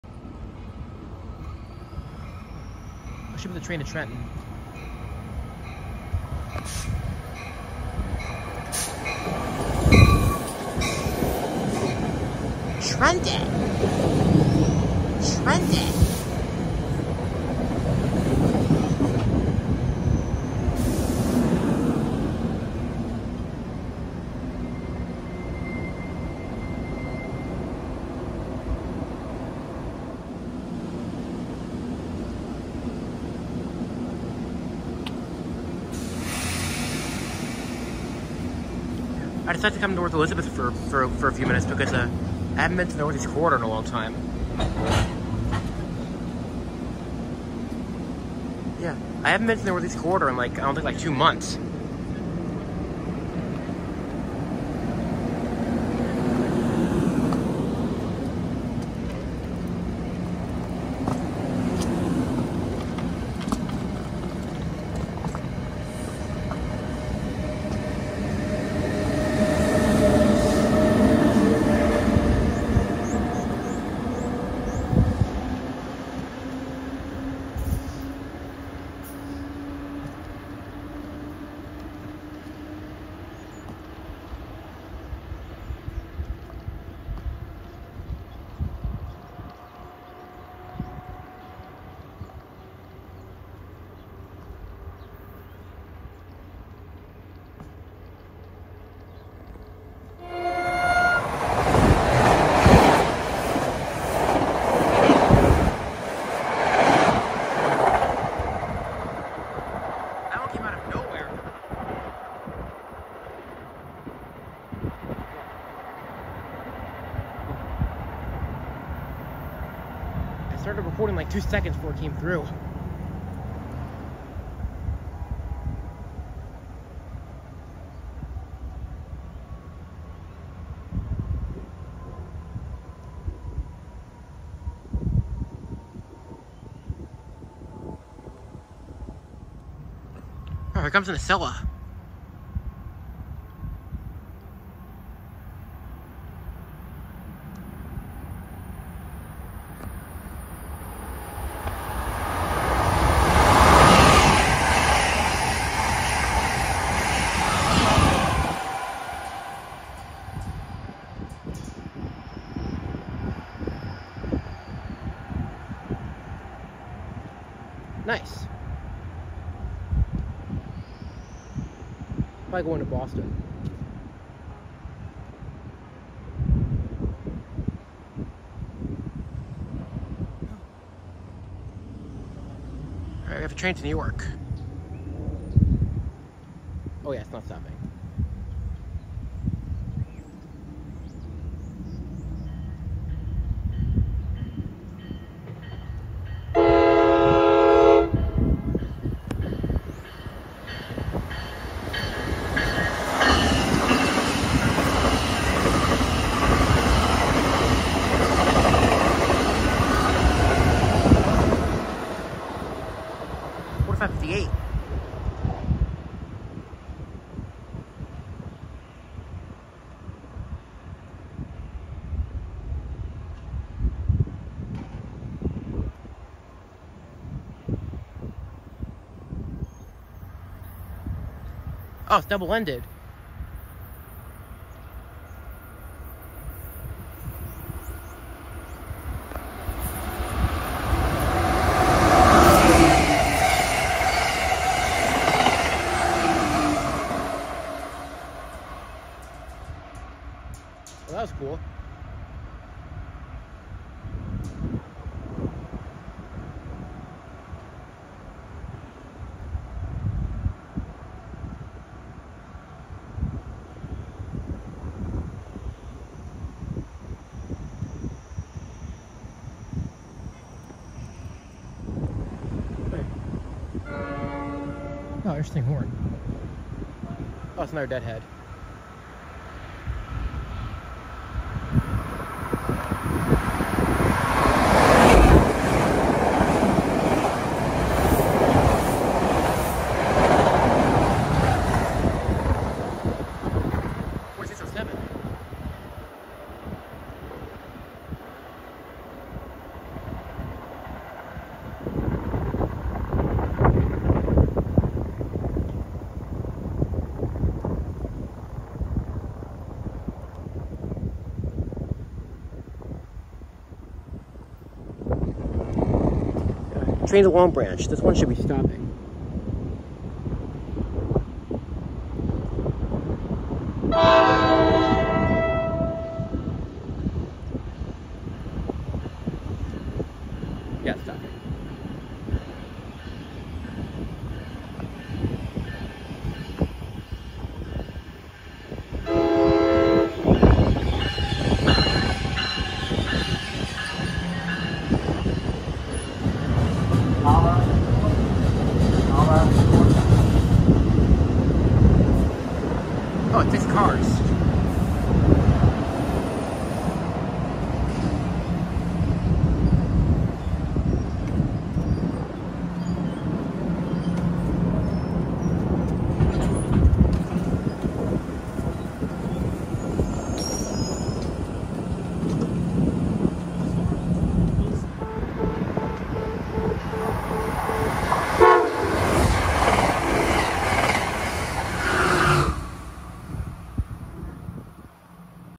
A few NJT and Amtrak trains at North Elizabeth, NJ 11/22/23
Last Wednesday after I watched the action at Union, I decided to head over to North Elizabeth for a few minutes see some of the action on the NEC since I haven’t been going to the NEC as much the past few months. I stayed for about half an hour and got to see quite a few trains!

1: NJT ALP-46 pushing a comet set to Trenton

2: NJT ALP-46A pulling a comet set to Long Branch

3: Amtrak Acela to Boston

4: NJT ALP-45A pushing a multilevel set to New York

5: Amtrak ACS-64 pushing a keystone to New York with another ACS-64 trailing

6: NJT ALP-46 pushing a comet set to Trenton

7: NJT ALP-45DP pulling a multilevel set to Long Branch

8: NJT ALP-46A I think pushing a multilevel set to New York

9: Amtrak ACS-64 pushing a keystone to Harrisburg